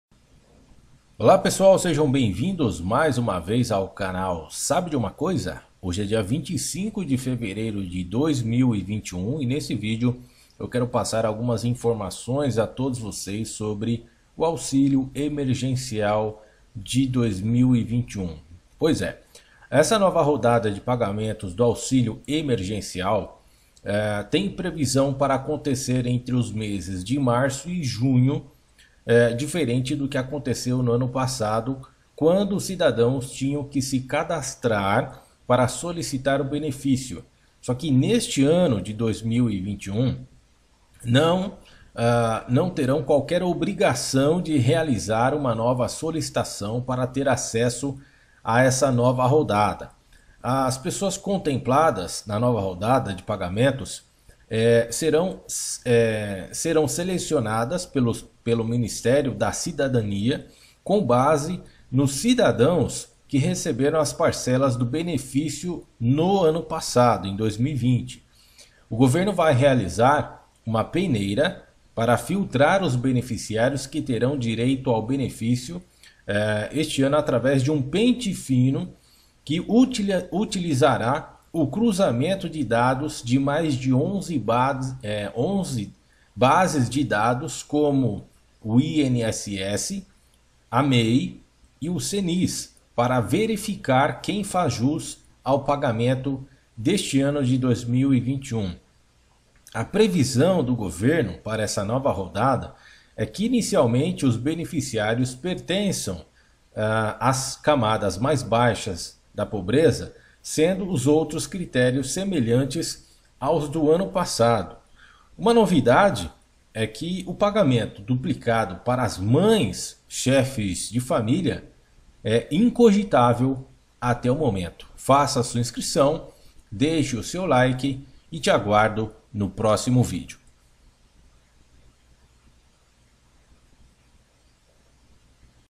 AUXILIO EMERGENCIAL 2021 E A NOVA RODADA DE PAGAMENTOS -QUANDO SERÁ?
A retomada do auxílio emergencial para informais na pandemia foi incluída em PEC cheia de temas polêmicos — incluindo o fim do gasto mínimo com saúde e educação em todas as esferas de governo.